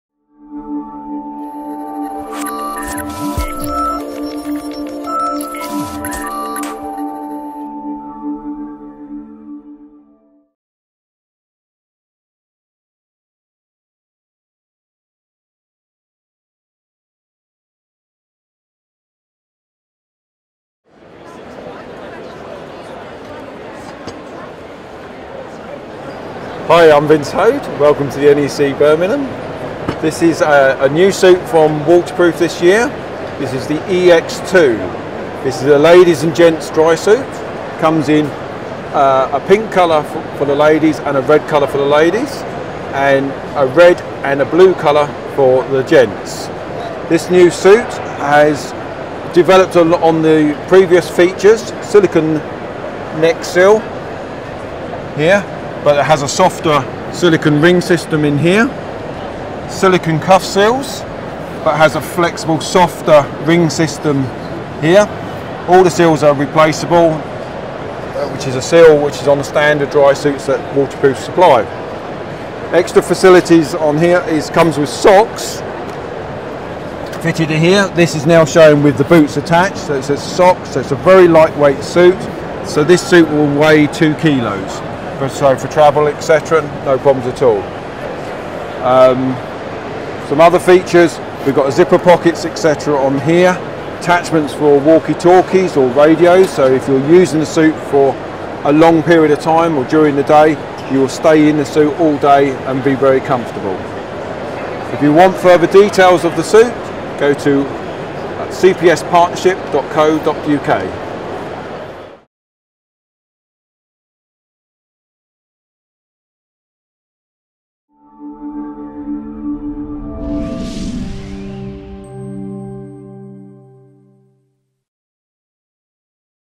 DIVE 2018 Review: Vince Hoad talks to Scubaverse about the new Waterproof EX2 Drysuit
In a video shot exclusively for Scubaverse.com at DIVE 2018, Vince Hoad from CPS Partnership talks to Scubaverse about the new Waterproof EX2 Drysuit.